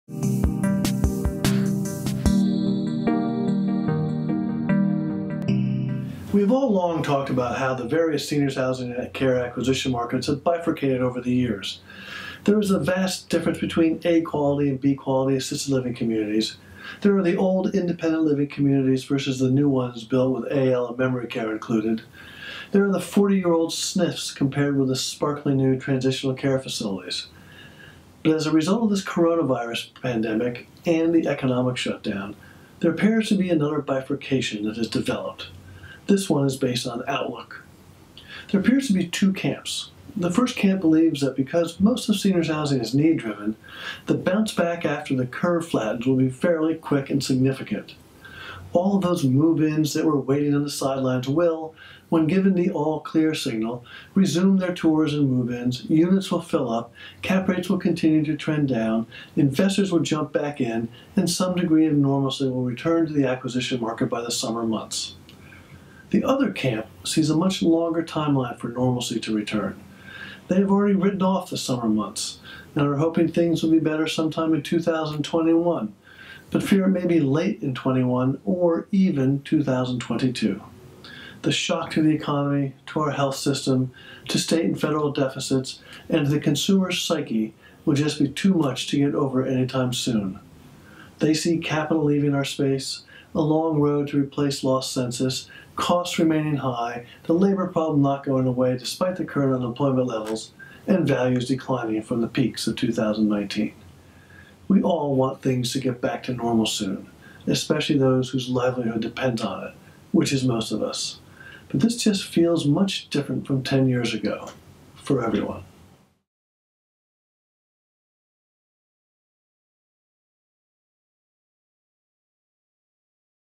The Bifurcating Seniors Housing Market - 60 Seconds 5.26.2020
The seniors housing and care acquisition market is bifurcating in more ways than one